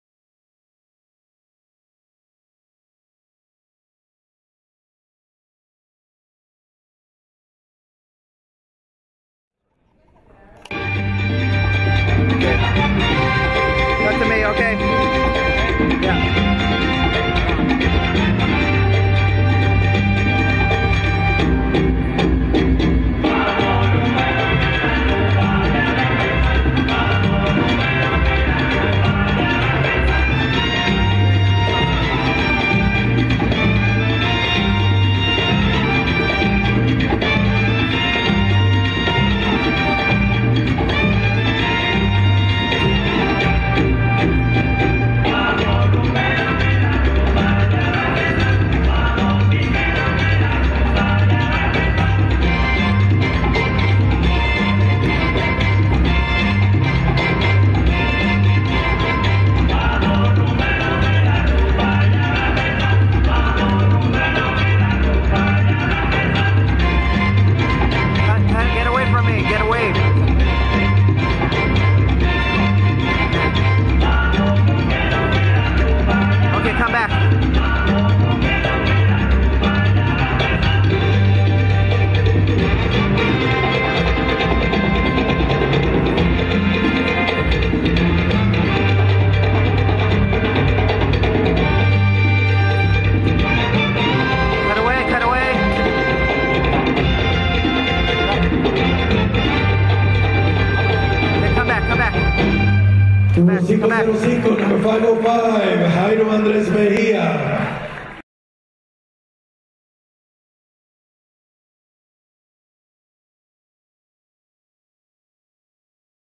Jairo Andres Mejia - Colombia - World Latin Dance Cup 2012 Soloist Kid -Semi Finals
Jairo Andres Mejia from Colombia competes at the World latin Dance Cup 2012 in Miami

Congratulations ! and See you at the 4th annual World Latin Dance Cup December 10th to 14th In Miami

SOLOIST KIDS SEMIFINALS SCORES:
KEVIN GALLEGO COLOMBIA 61.19
JORDYNN LURIE USA 59.75
BRANDON AYALA PORTLAND, USA 58.81
GIAN NICOLAS VARELA 56.75
JUAN CAMILO ROZO COLOMBIA 56.50
CATALINA RUEDA COLOMBIA 52.44
JAIRO ANDRES MEJIA COLOMBIA 45.56